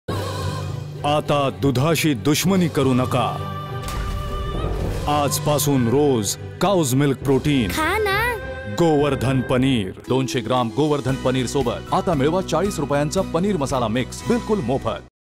Gowardhan Paneer TVC - 15s - Marathi
बहुतेक मुलांना दूध आवडत नाही. आता काळजी नाही! गोवर्धन पनीरच्या माध्यमातून तुमच्या मुलांना दररोज त्यांचे प्रथिने आणि कॅल्शियम द्या. खरेदी करा २०० ग्रॅम गोवर्धन पनीर व त्यासोबत मोफत मिळवा रु. ४० किमतीचे पनीर मसाला मिक्स.